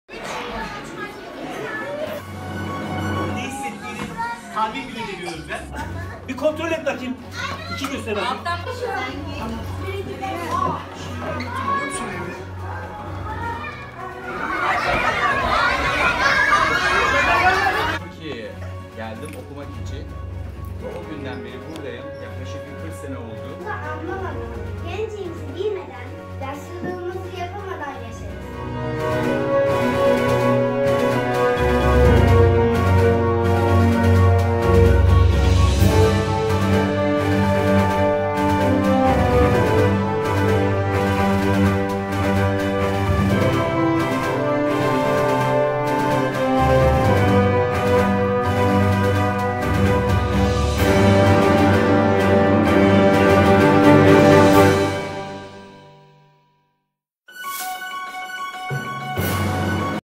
FARKLI MODELDE BÜYÜKANNE BÜYÜKBABA GÜNÜ
BÜYÜKANNE BÜYÜKBABA GÜNÜ
Okulumuz öğrencileri yaşlılar haftası etkinlikleri kapsamında büyükanne ve büyükbabalarını okullarında ağırladılar. Bu güzel etkinlikte bizlerin yanında olan tüm velilerimize teşekkür ediyoruz.